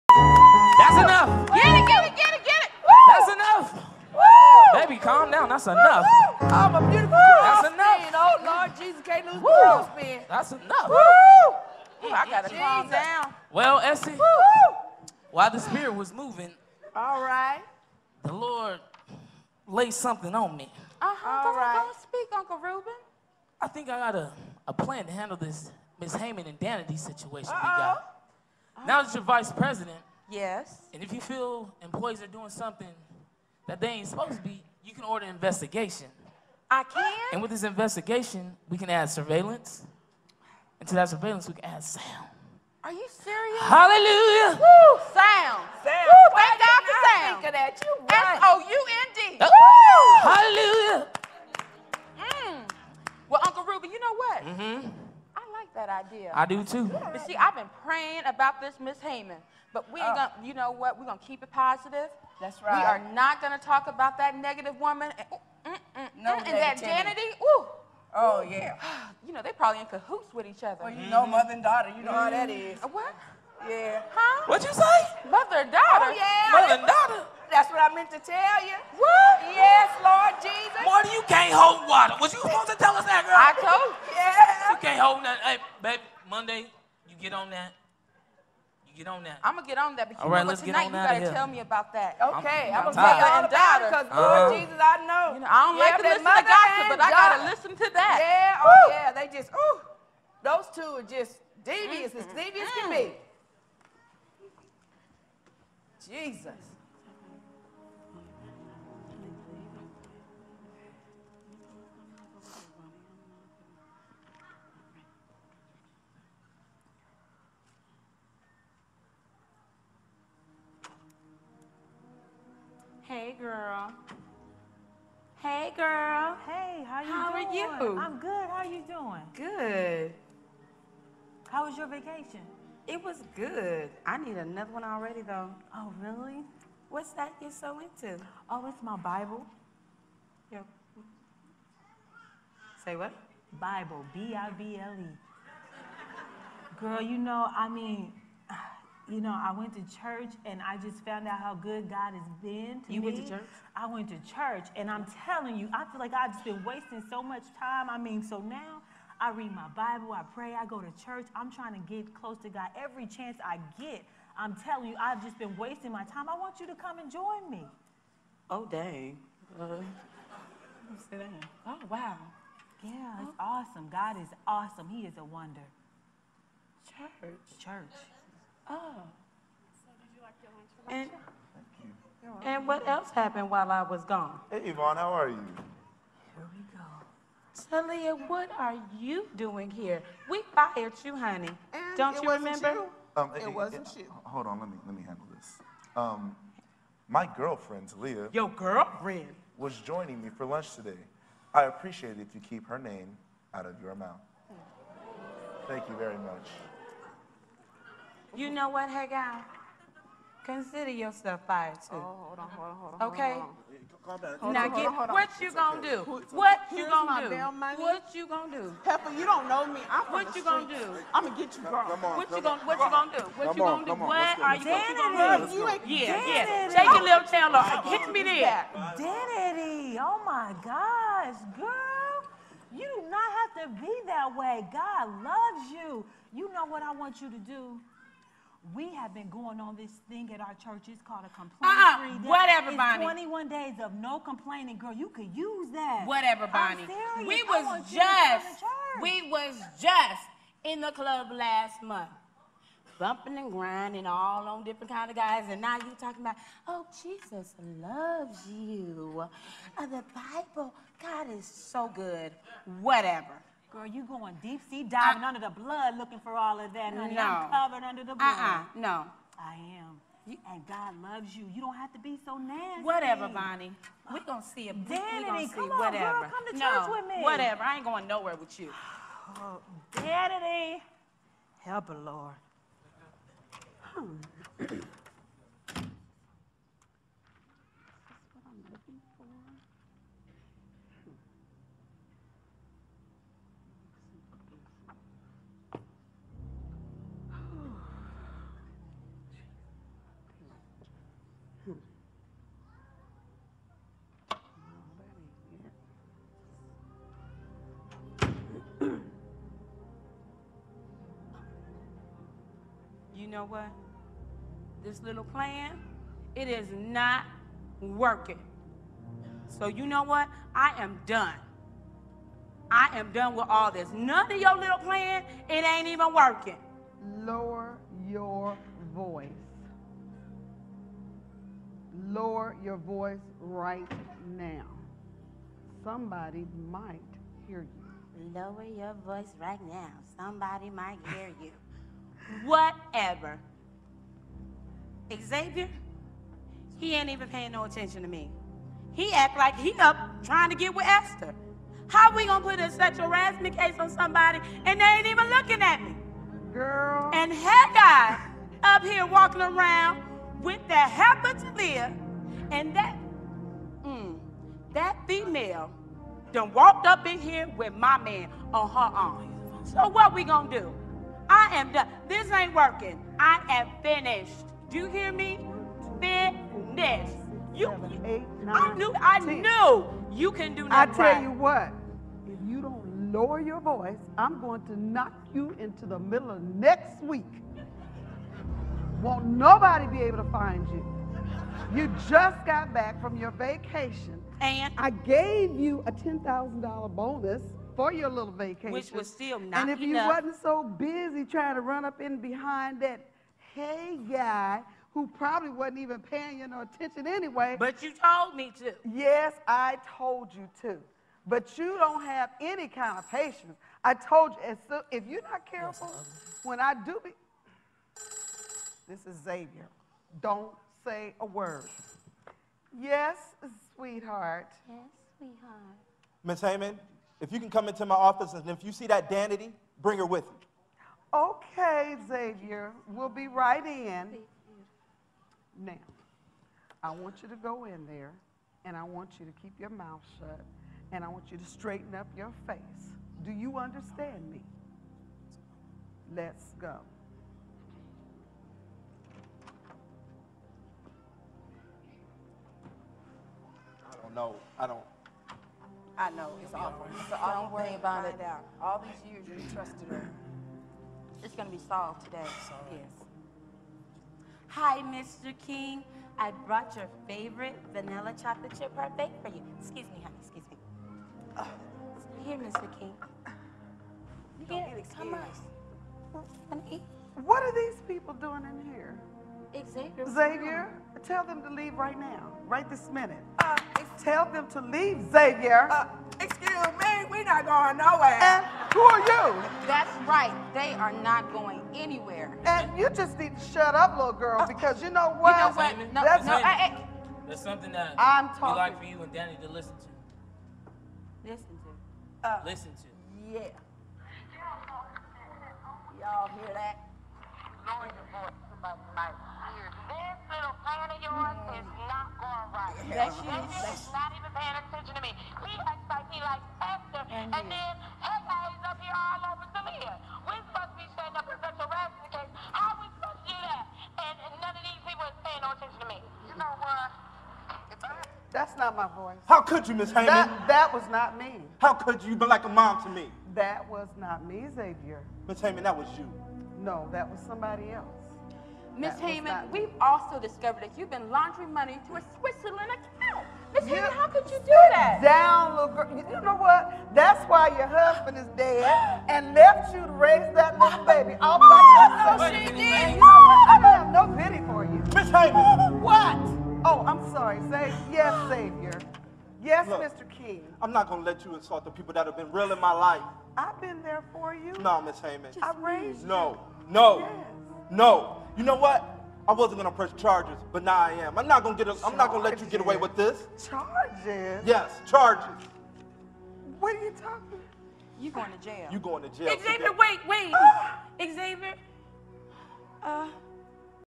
Esther's Journey Stage Play Part 9
A widowed mother-to-be, Esthers triumph over grief, loss and deception is a true testament to the power of  faith and Gods love.  The stage is set for drama that plunges Esther into a whirlpool of twisted family ties, moral situations, and betrayal that threatens to destroy the company. With an unforgettable heroine and a vividly evocative portrayal of a strong family, ESTHERS JOURNEY rivets with passion and power. Written and Directed by: Nicole Newman-Colter